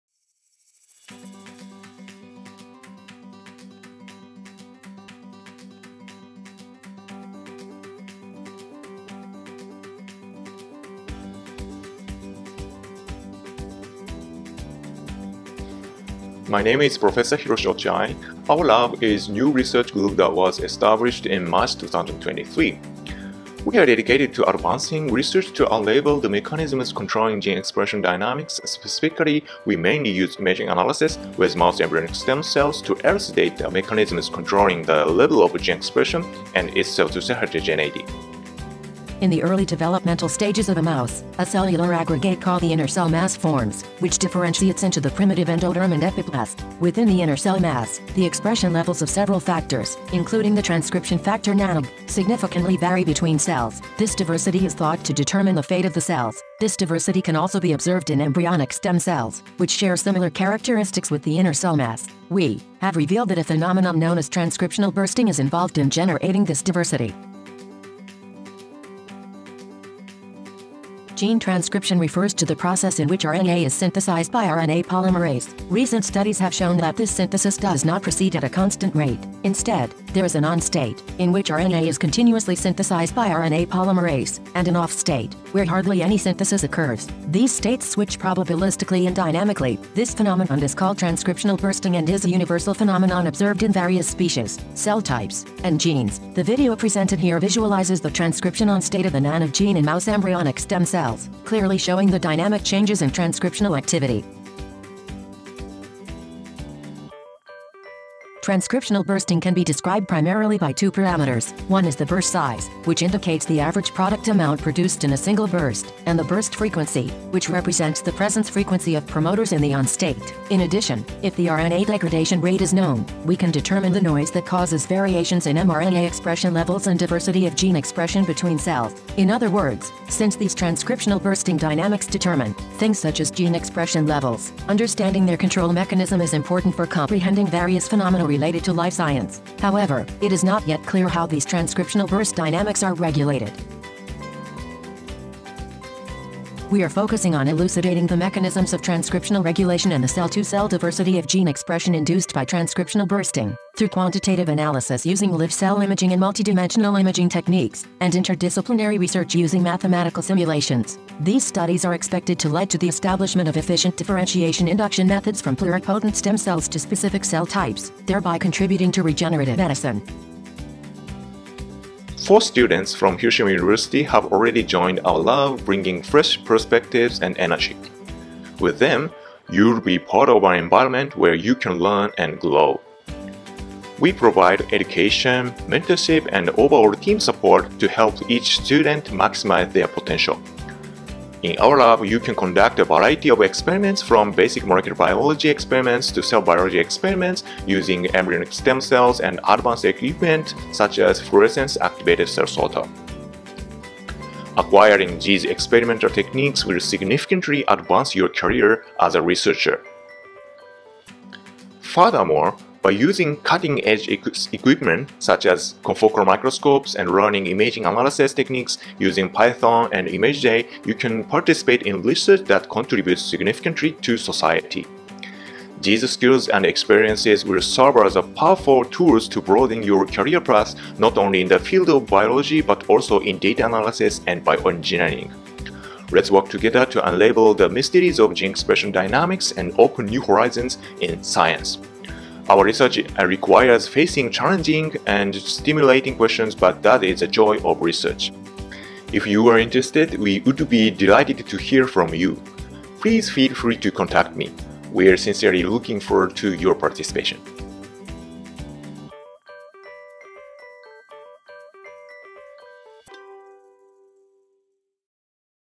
【生命医科学】遺伝子発現動態学（落合）研究室紹介動画
九州大学システム生命科学府の生命医科学分野遺伝子発現動態学（落合）研究室についての紹介動画です。